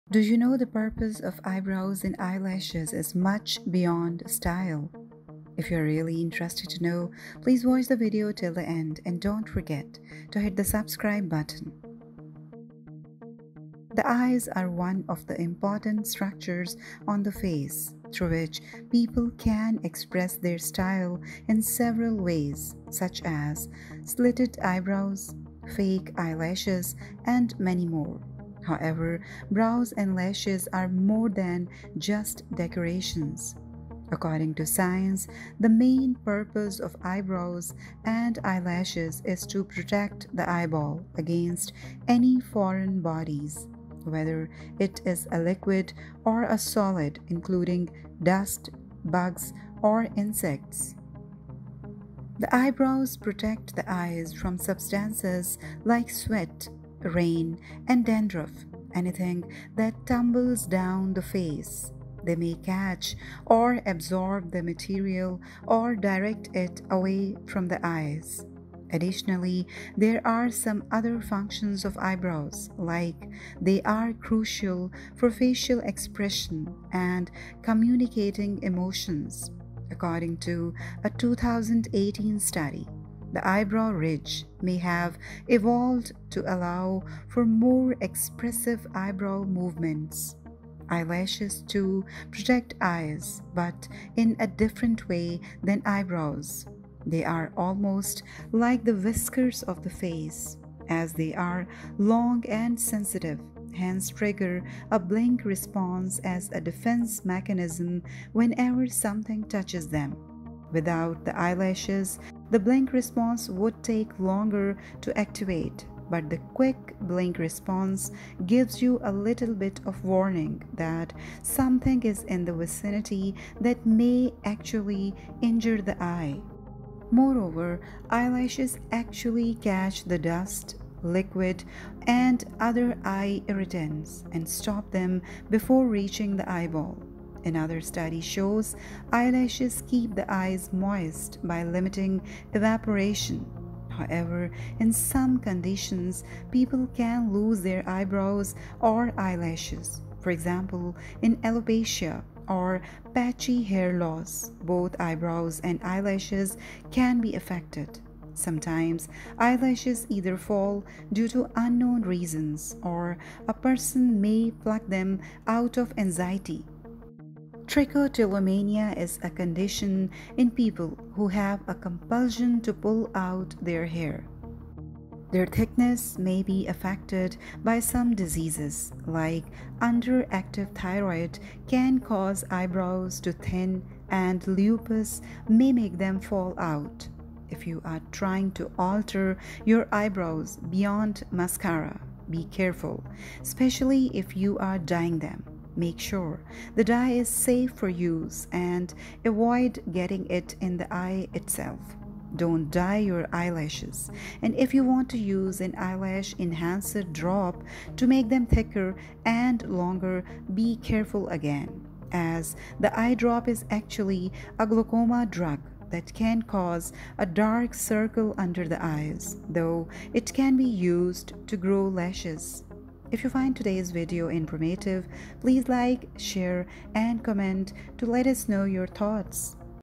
Why Do We Have Eyelashes? - What is the purpose of your Eyelash ? Science Count
The role of the eyelashes
Even though they are often regarded as an emphasis of beauty, their primary function is to protect the eyes from small particles such as dust, sand or debris from entering and harming the eye. ... So, you can think of the eyelashes as the protective 'gatekeeper' of the eye.

Our Website for Daily Updates.